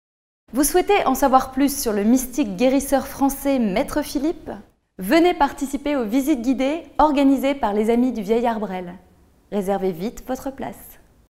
visite guidée maître Philippe à l’Arbresle
Vous souhaitez en savoir plus sur le mystique guérisseur français Maître Philippe ?
Venez participer aux visites guidées organisées par « Les amis du Vieil Arbresle » !
Réservez vite votre place pour une visite guidée à la découverte de maître Philippe à l’Arbresle !

"Aidez Sofian dans son combat contre l'ataxie telangiectasie"